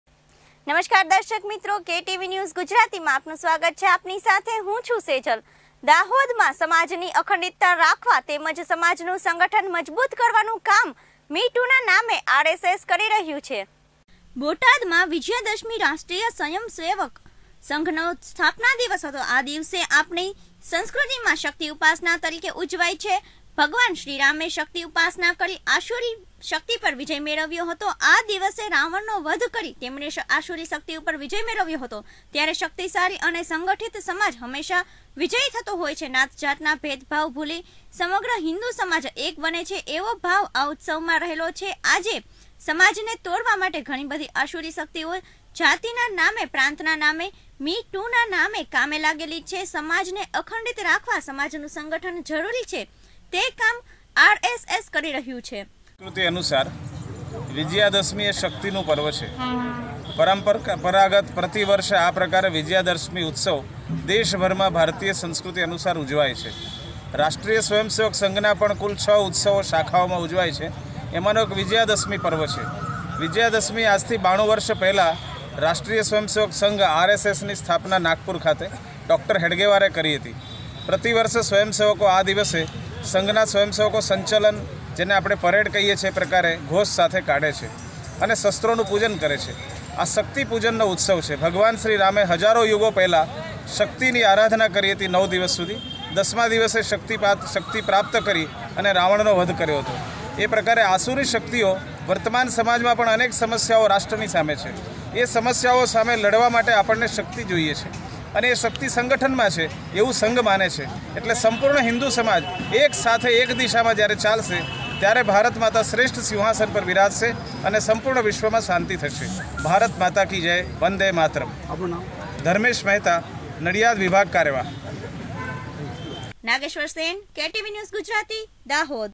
દાહોદમાં સમાજની અખંડિતતારાખવા તેમજ સમાજનું સંગઠન મજબૂત કરવાનું કામ "મી ટુ"ના નામે
દાહોદમાં સમાજની અખંડિતતારાખવા તેમજ સમાજનું સંગઠન મજબૂત કરવાનું કામ "મી ટુ"ના નામે આર. એસ.એસ. કરી રહ્યું છે